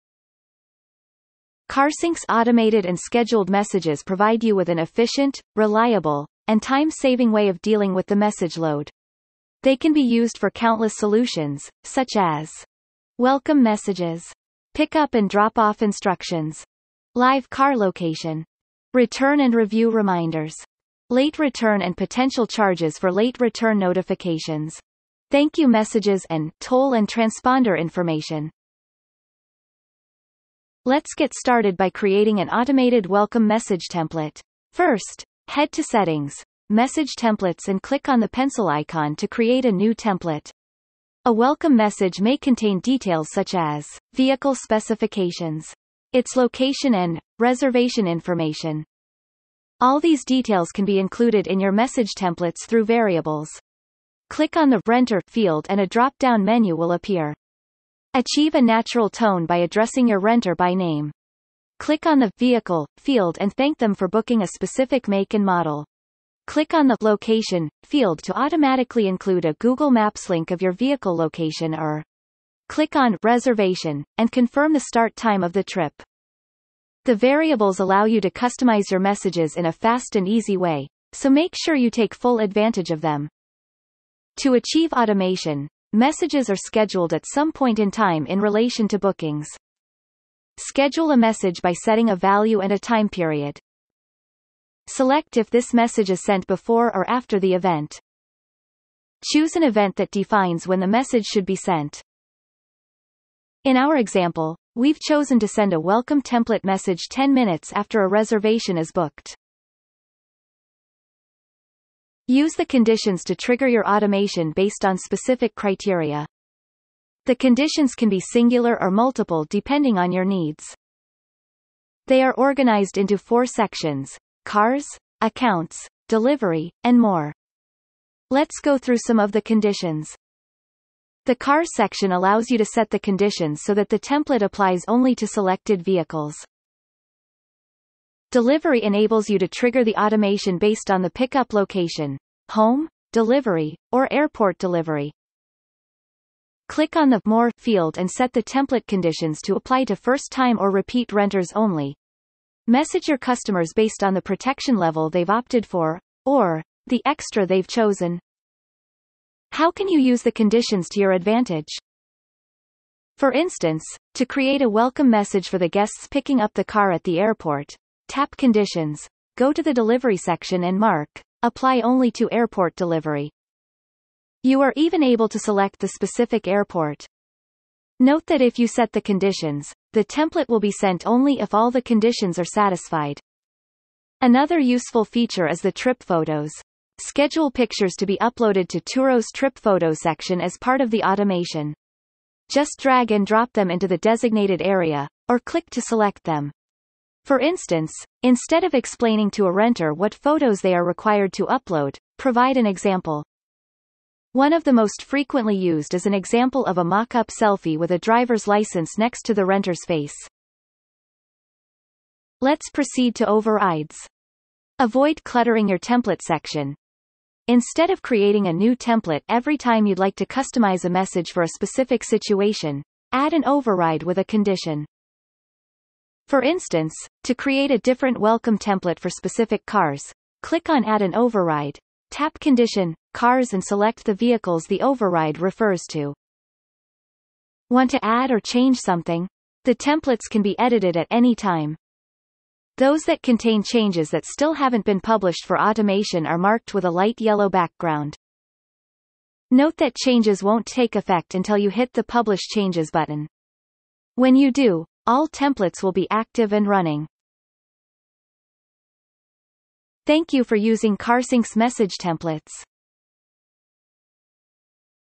How to set up the CarSync Automated messages
CarSync’s automated and scheduled messages provide you with an efficient, reliable, and time-saving way of dealing with the message load. They can be used for countless solutions, such as:

► Welcome messages,
► Pick-up and drop-off instructions,
► Live car location,
► Return and review reminders,
► Late return and potential charges for late return notifications,
► Thank-you messages
► and Toll and transponder information.

Let’s get started by creating an automated welcome message template!

Hi {renter_full_name},

Thank you for renting {vehicle_make} {vehicle_model}. This car uses {vehicle_fuel_type} fuel.

This car is ideal for the mountain terrain with its all-wheel drive.

The car location is {reservation_location}.

Your trip starts at {reservations_start_time}. You will receive detailed instructions one hour before the trip starts.

First, head to Settings | Message Templates and click on the pencil icon to create a new template.

VARIABLES

A welcome message may contain details such as:
► Vehicle specifications,
► Its location
► and Reservation information.

All these details can be included in your message templates through variables:
► Click on the “Renter” field and a drop-down menu will appear. Achieve a natural tone by addressing your renter by name,
► Click on the “Location” field to automatically include a Google Maps link of your vehicle location or
► Click on “Reservation” and confirm the start time of the trip.

The variables allow you to customize your messages in a fast and easy way, so make sure you take full advantage of them.

SCHEDULING

To achieve automation, messages are scheduled at some point in time in relation to bookings.

Schedule a message by setting a value and a time period.
Select if this message is sent Before or After the event.
Choose an event that defines when the message should be sent.

In our example, we’ve chosen to send a welcome template message 10 minutes after a Reservation is booked.

CONDITIONS

Use the conditions to trigger your automation based on specific criteria. The conditions can be singular or multiple depending on your needs.

They are organized into four sections: Cars, Accounts, Delivery, and More. Let’s go through some of the conditions:
► The Cars section allows you to set the conditions so that the template applies only to selected vehicles.
► Delivery enables you to trigger the automation based on the pick-up location: home, delivery, or airport delivery.
► Click on the “More” field and set the template conditions to apply to first-time or repeat renters only, message your customers based on the protection level they’ve opted for, or, the extra they’ve chosen.

How can you use the conditions to your advantage?
For instance, to create a welcome message for the guests picking up the car at the airport, tap Conditions, go to the Delivery section and mark “Apply only to airport delivery”. You are even able to select the specific airport.

Note that if you set the conditions, the template will be sent only if all the conditions are satisfied.

TRIP PHOTOS

Another useful feature is the Trip Photos. Schedule pictures to be uploaded to Turo’s trip photos section as part of the automation. Just drag and drop them into the designated area, or click to select them. For instance, instead of explaining to a renter what photos they are required to upload, provide an example. One of the most frequently used is an example of a mock-up selfie with a driver’s license next to the renter’s face.

OVERRIDES

Let’s proceed to overrides! Avoid cluttering your template section - instead of creating a new template every time you’d like to customize a message for a specific situation, add an override with a condition.

For instance, to create a different welcome template for specific cars, click on Add an override, tap Condition | Cars and select the vehicles the override refers to.

Want to add or change something? The templates can be edited at any time.
Those that contain changes that still haven't been published for automation are marked with a light yellow background.

Note that changes won't take effect until you hit the PUBLISH CHANGES button.
When you do, all templates will be active and running.

Thank you for using CarSync’s Message Templates!